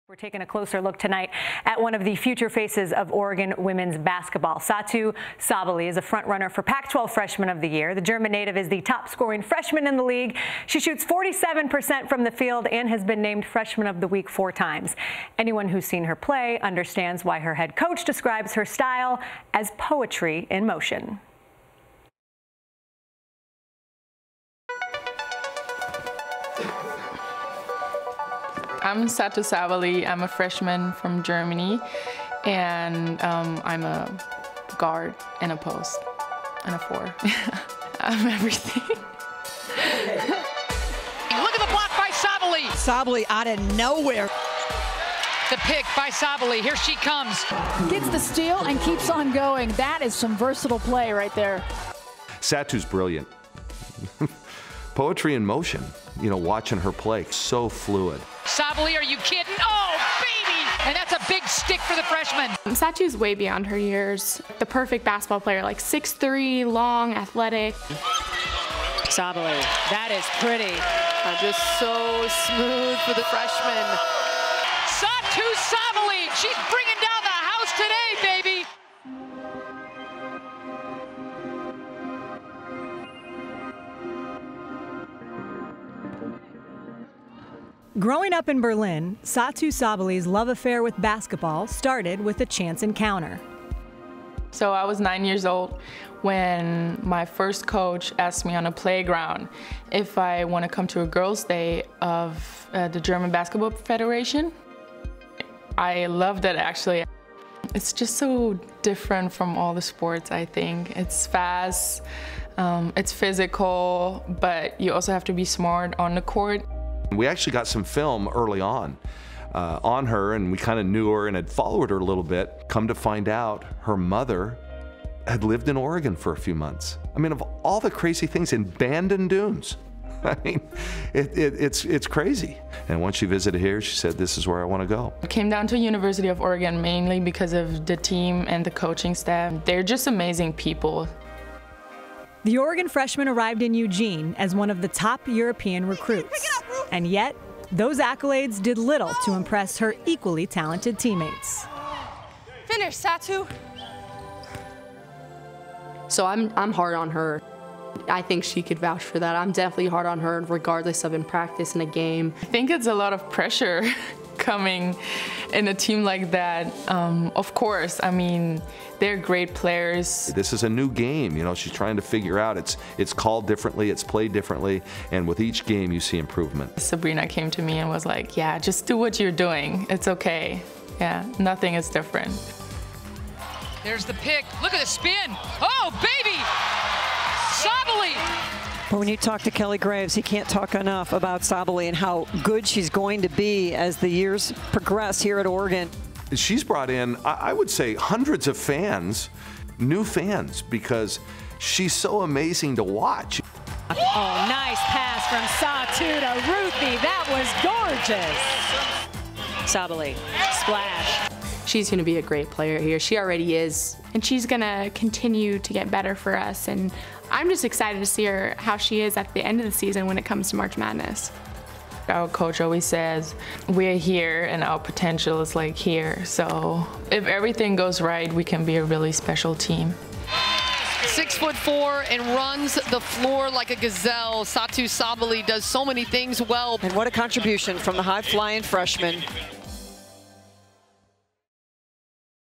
How Satou Sabally translates her uncanny basketball ability to big production for No. 8 Oregon
A truly positionless player who hails from Berlin, freshman Satou Sabally has burst onto the Pac-12 women's basketball scene with authority this season while providing the No. 8 Ducks crucial production during a historic season.